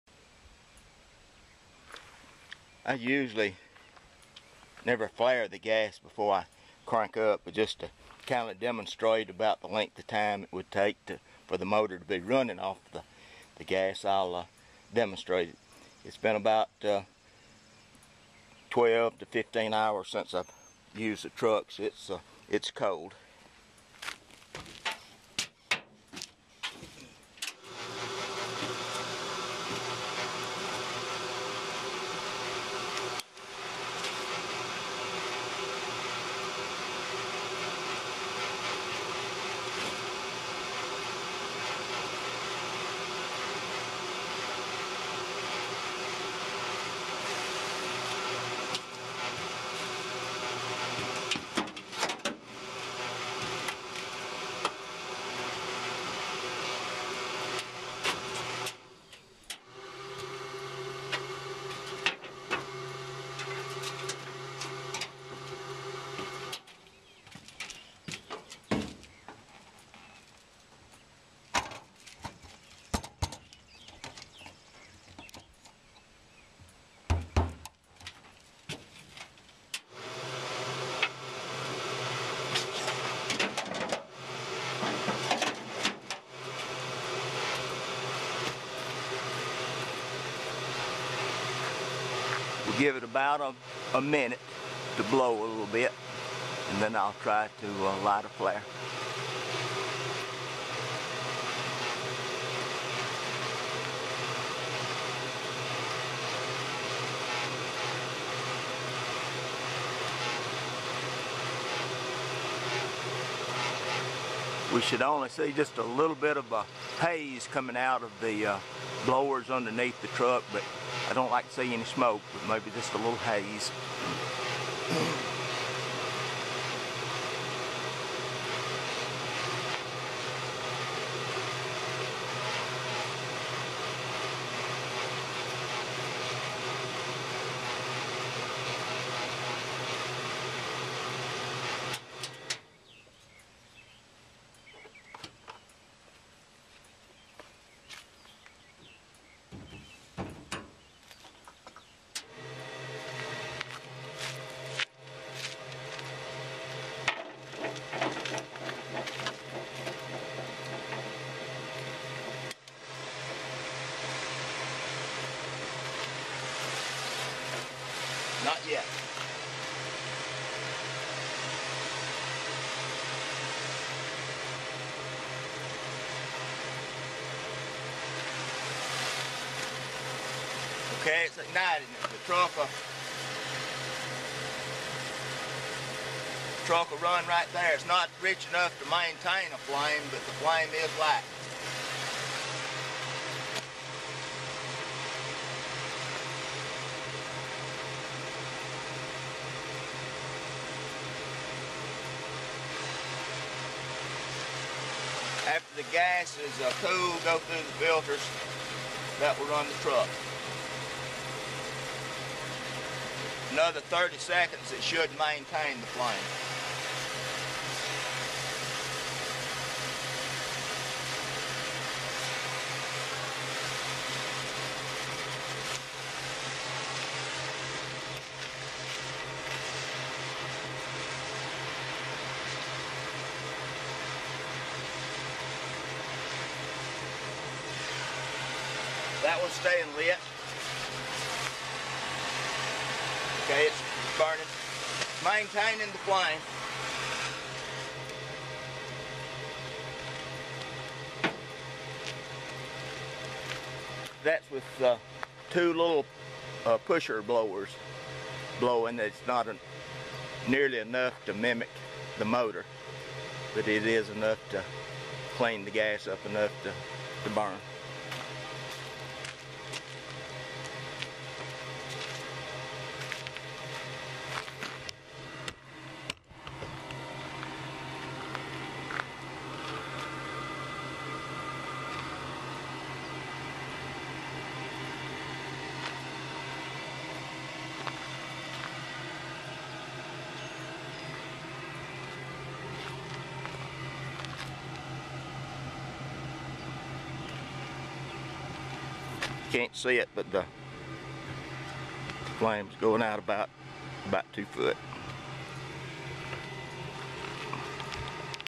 gasification , cold gas flare 92 dakota
Showing length of time from light off that good gas is being made.